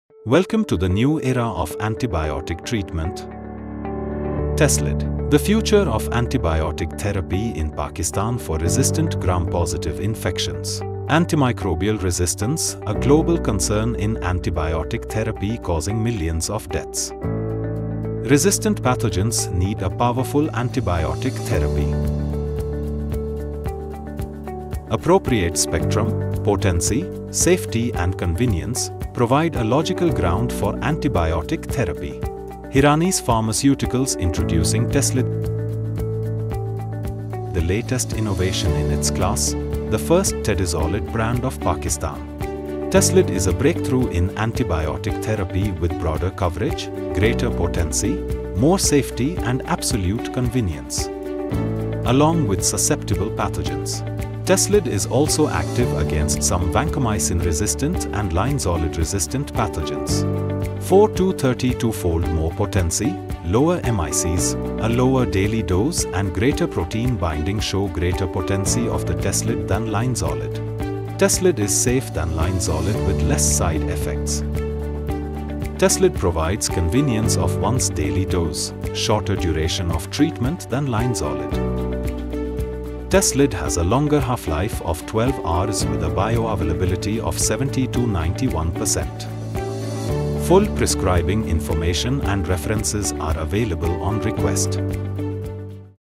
Tezlid (Tedizolid) Oxizolidenone Antibiotic.
Pakistan First Tedizolid Barand Tezlid 200mg is now available all over the Pharmacies.

(Tedizolid phosphate)
TEZLID is a second generation oxazolidinone antibiotic and Tezlid tablets are indicated for the treatment of acute bacterial skin and skin structure infections (ABSSSI) caused by susceptible strains, in adults 12 years of age and older. The investigational used include bone and joint infections, diabetic foot infections, hospital acquired bacterial pneumonia, pneumococcal bacteremia and VRE faecium bacteremia.
Full Prescribing information is available on request.